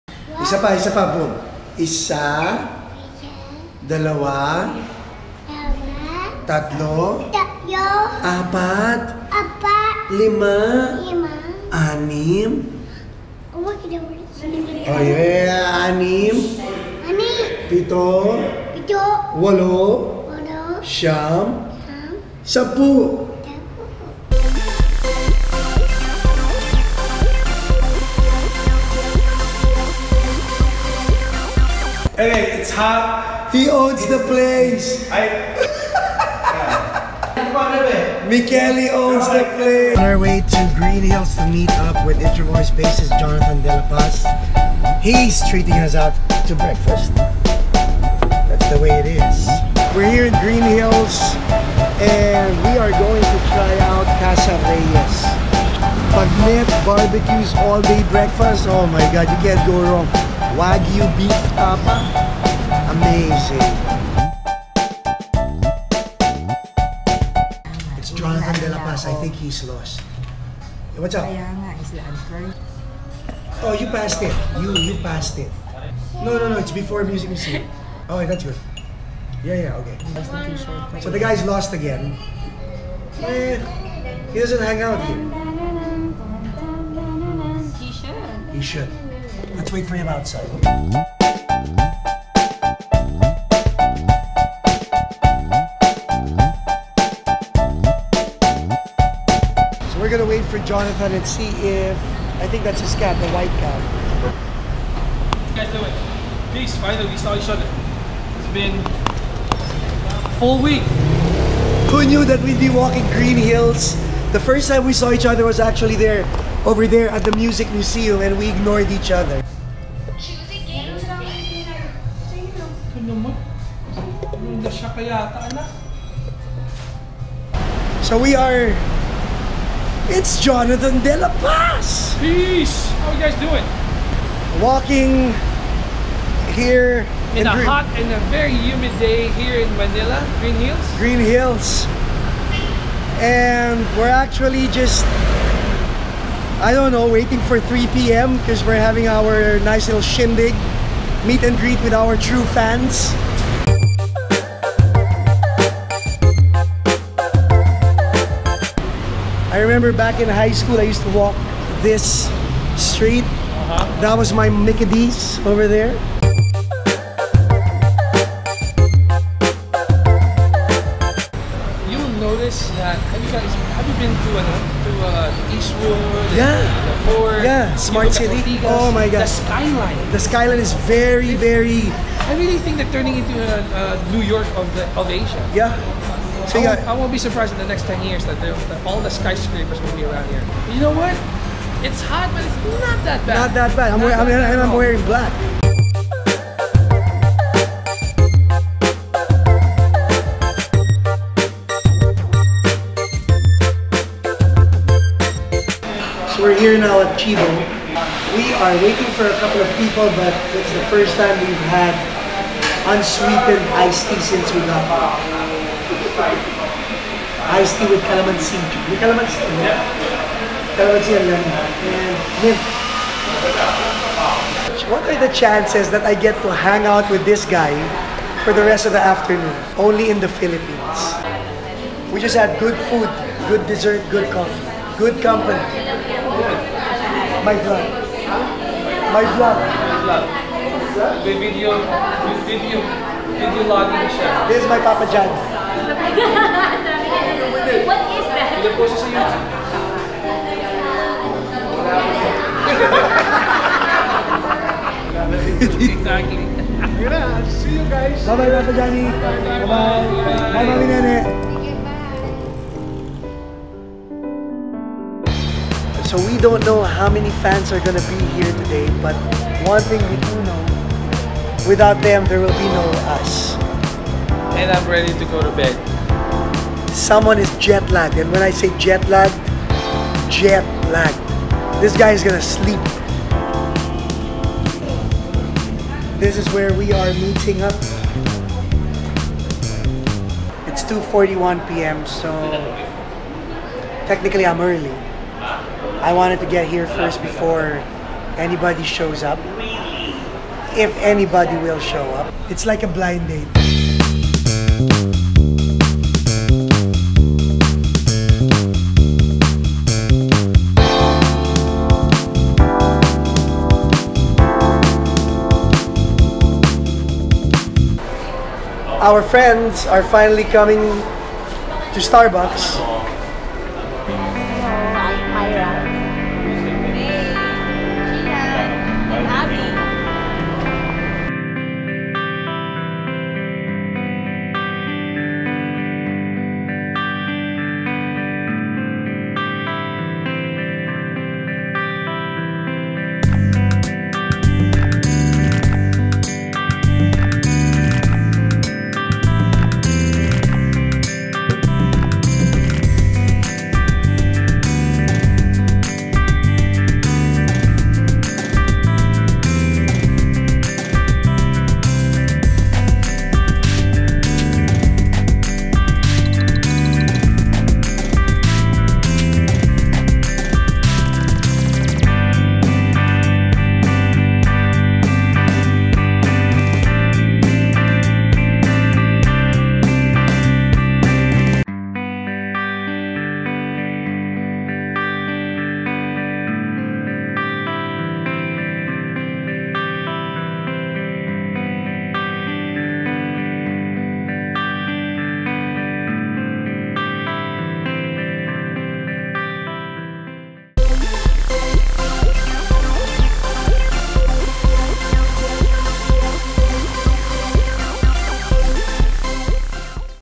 Let's go to Greenhills | Its more fun in the Philippines - Vlog#19
Greenhills, in the city of Mandaluyong is special to me. I have very fond memories of the place. It was the first "mall" I ever hung out at. Fist fights and brawls happened here. And weekend drag races and car showcases on Saturday nights. Who wouldn't want to see Greenhills again?

Anyway, on April 26, 2017... it was time to walk the curbs of Greenhills once again, not only to relive the memories but to meet up with important people.

I've always wanted to be an artist. And I knew how to start a band. But in order to be a successful artist, you have to know how to build your fan base. And mind you, our fans are loyal, not because we demand them to, but because they choose to be so.

And the reason they are is because with treat all of them with love and respect and they know that we, INTRoVOYS, are always grateful to them for investing their time and money in our music.

Today, I was contemplating where to meet up with my fans. It was starbucks vs coffee bean. Of course, Starbucks won!

This blog episode is dedicated to all of our True Fans around the world. It is my desire that one day, we can fill an arena and celebrate our relationship, even for just one day.

Again, it's more fun in the Philippines